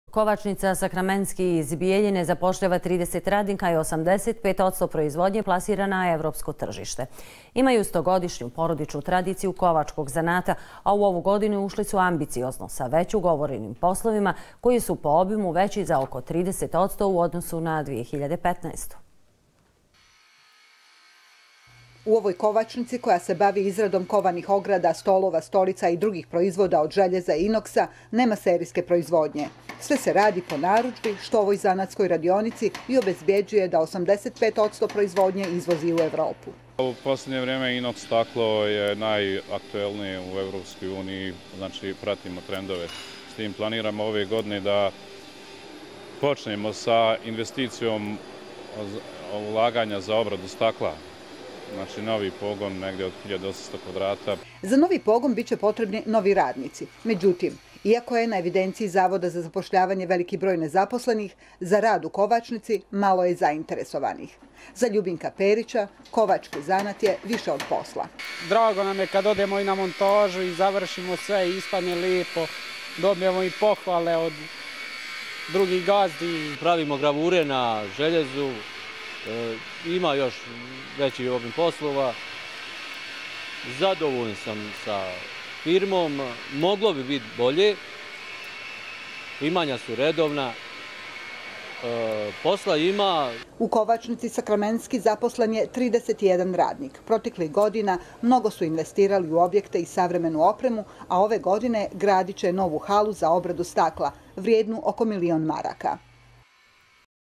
Bijeljina: Kovačnica "Sakramentski" uspješno posluje na evropskom tržištu
Kovačnica "Sakramentski" iz Bijeljine zapošljava trideset radnika i 85 odsto proizvodnje plasira na evropsko tržište.